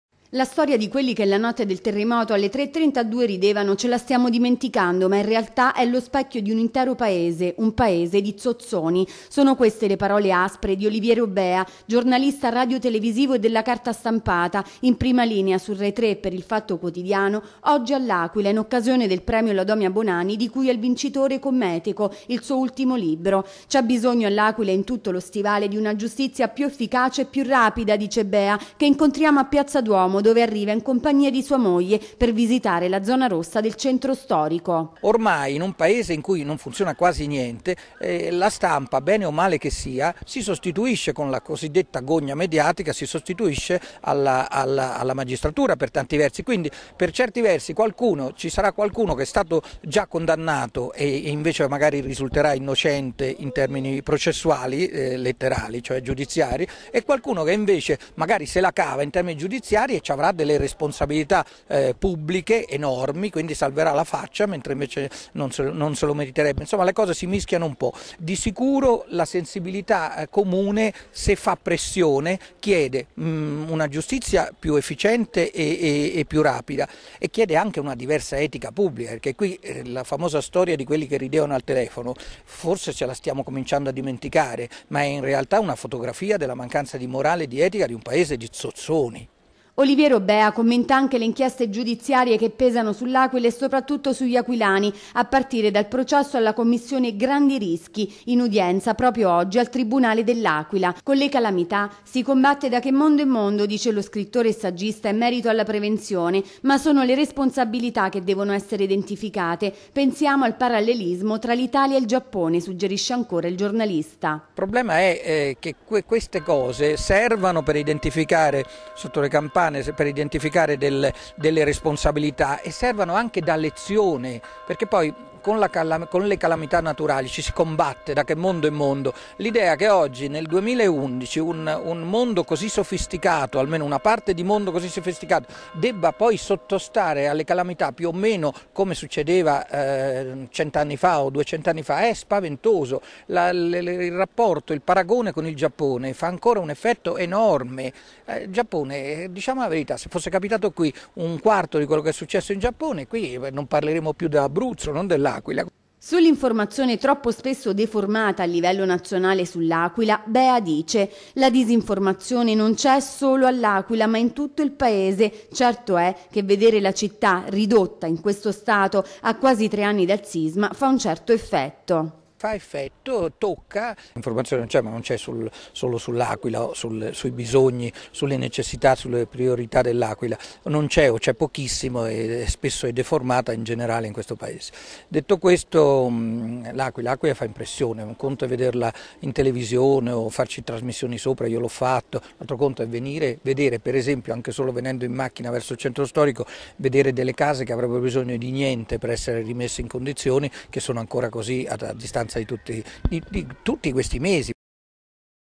TVUNO: OLIVIERO BEHA A L'AQUILA
TVUNO L'AQUILA - "L'Aquila è lo specchio del paese. Un paese di zozzoni." Il giornalista Oliviero Beha a L'Aquila in occasione del premio Bonanni di cui è il vincitore con Meteko il suo ultimo libro, commenta la situazione in cui versa la città e il processo alla Commissione Grandi Rischi.